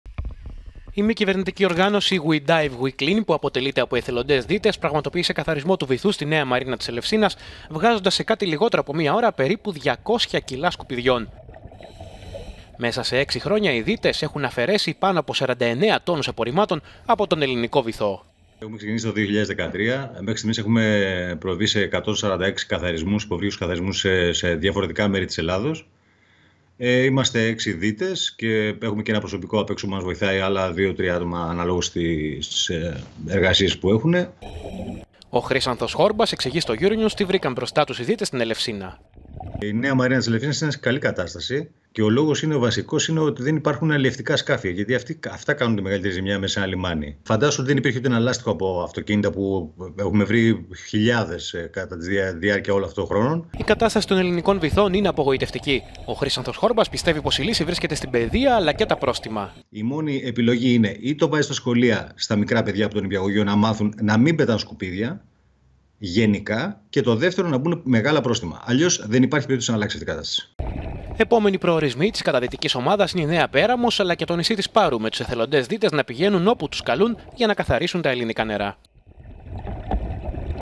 Καθαρισμός του βυθού στην Ελευσίνα-200 κιλά σκουπίδια αφαιρέθηκαν(vid)…
Η ΜΚΟ "We dive We clean" καθάρισε τη Νέα μαρίνα Ελευσίνας …